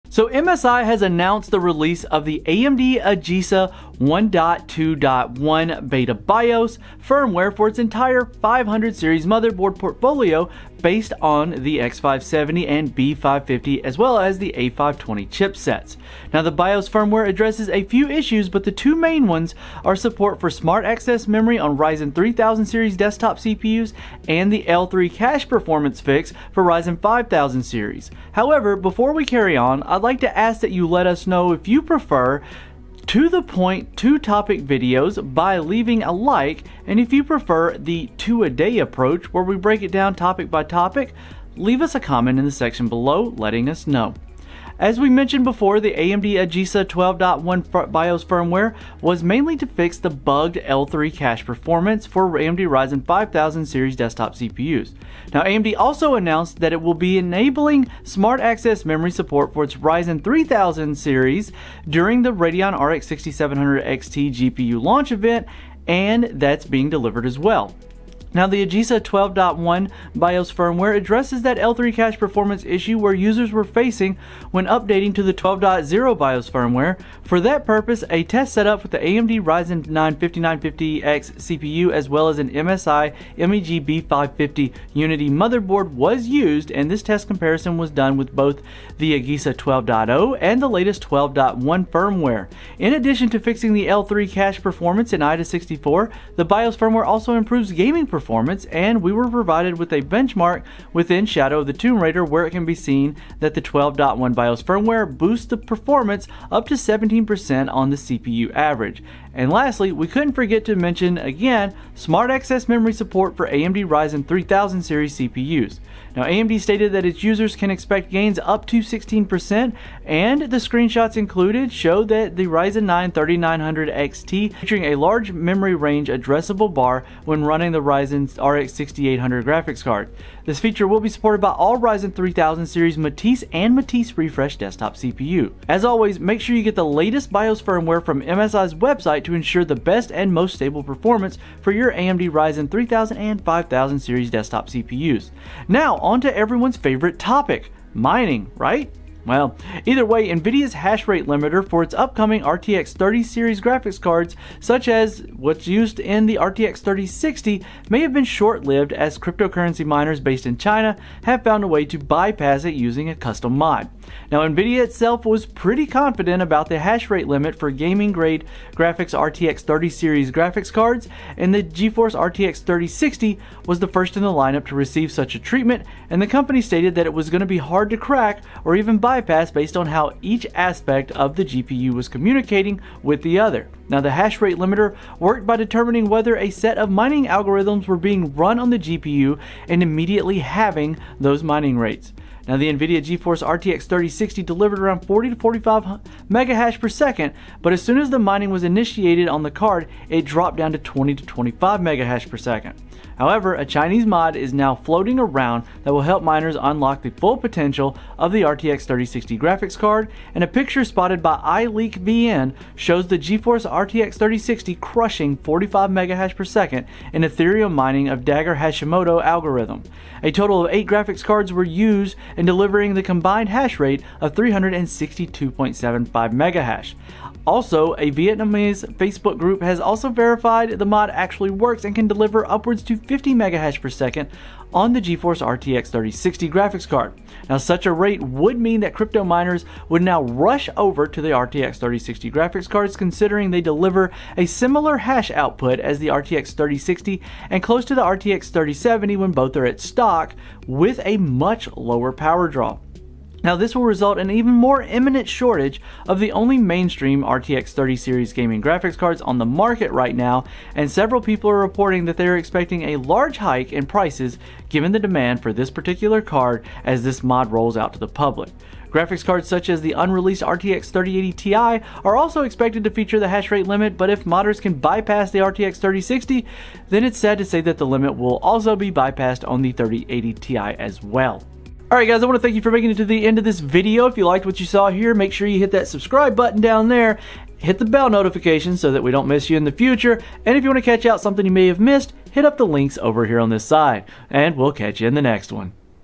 MSI Unleashes SAM On Ryzen 3000! NVIDIA Hash Limiter Claimed To Be Hacked?
MSI has officially announced the release of the AMD AGESA 1.2.0.1 BETA BIOS Firmware for its entire 500-series motherboard portfolio based on the X570, B550, and A520 chipsets. NVIDIA's hash rate limiter for its upcoming GeForce RTX 30 series graphics cards seemingly already have a workaround [No, no it doesn't, The Hash Limiter is still in place]